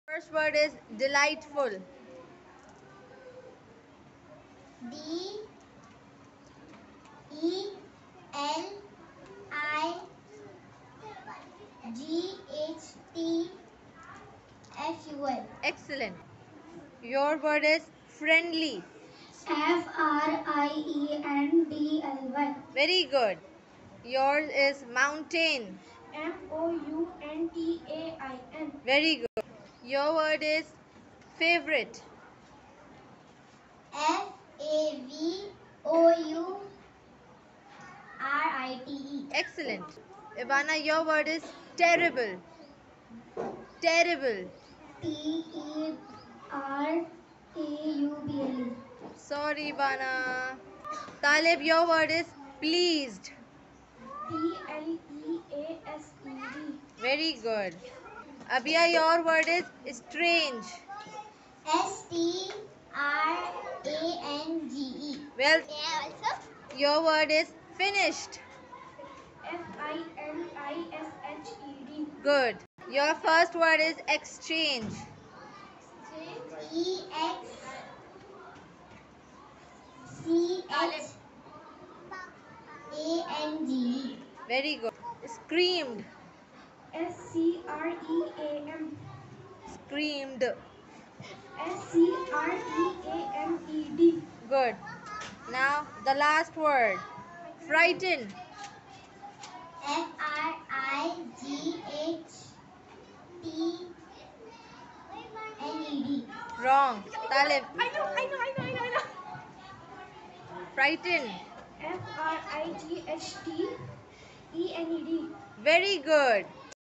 November 10, 2023(1)
🔠🏆 Spelling_Competition vibes!

Watch these brilliant students of Junior_III showcase their spelling skills and embrace the power of words.

Aim: It helps in enhancing communication and building confidence.